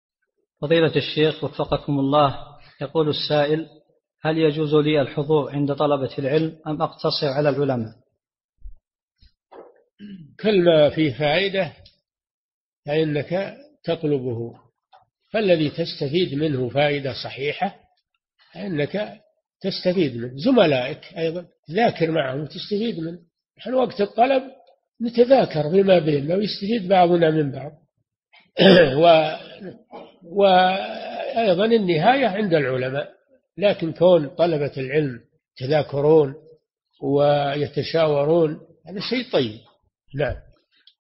[139 -308] هل أطلب العلم عند طلاب العلم أم أقتصر على العلماء فقط؟ - الشيخ صالح الفوزان
-[139 -308] السؤال: هل يجوز لي الحضور عند طلبة العلم أم أقتصر على العلماء؟
الجواب: كلما فيه فائدة فإنك تطلبه، فالذي تستفيد منه فائدة صحيحة، فإنك تستفيد منه زملائك أيضًا ذاكر معهم أيضًا وتستفيد منهم، نحن وقت الطلب نتذاكر فيما بيننا ويستفيد بعضنا من بعض، وأيضًا النهاية عند العلماء، لكن كون طلبة العلم يتذاكرون، ويتشاورون هذا شيء طيب.
للاشتراك في قناة الشيخ صالح الفوزان

للاستفسار والإقتراحات والدعم
رئيس الفريق
لمشروع كبار العلماء